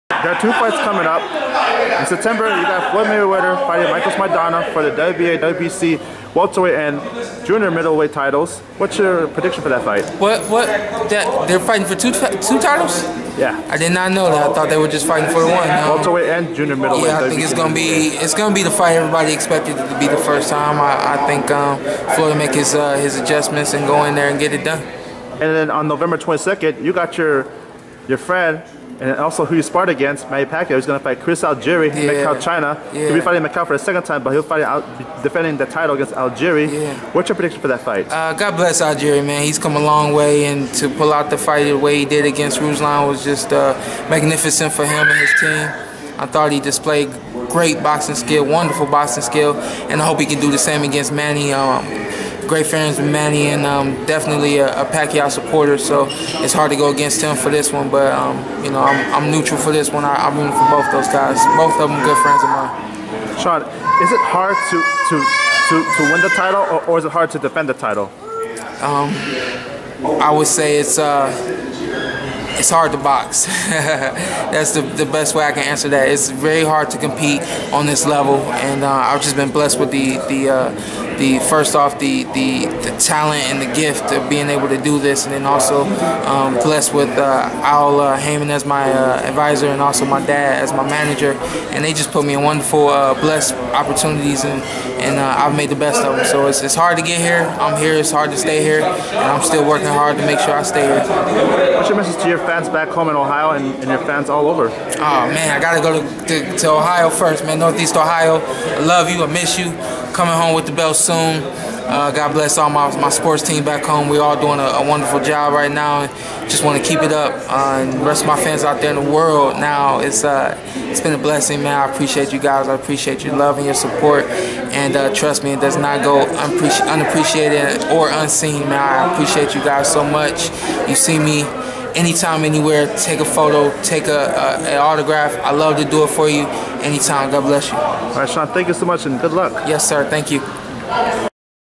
Shawn Porter: Talks Mayweather-Maidana 2, "Pacquiao-Algieri is 50-50 and it is hard to box."
Shawn Porter breaks down Mayweather-Maidana 2, and Pacquiao Algieri. Plus, I produce episodes of The Hangout.